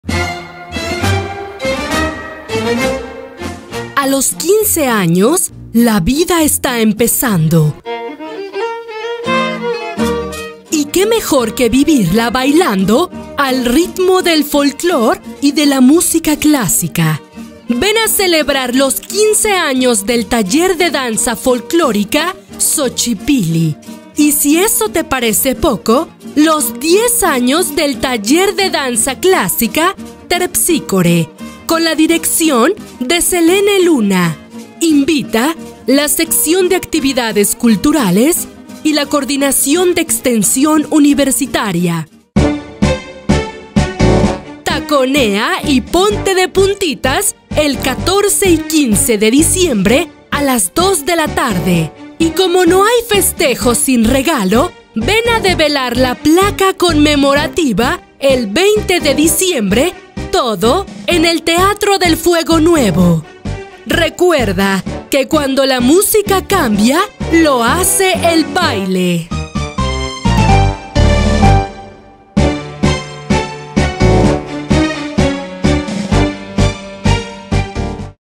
XOCHIPILLI CUMPLE XV AÑOS Y TERPSÍCORE X
Taconea y ponte de puntitas el 14 y 15 de diciembre a las 2 de la tarde para festejar los Talleres de Danza  Xochipilli y Terpsícore de la UAM-Iztapalapa. Y como no hay festejo sin regalo, ven develar la placa conmemorativa el 20 de diciembre, en el Teatro del Fuego Nuevo.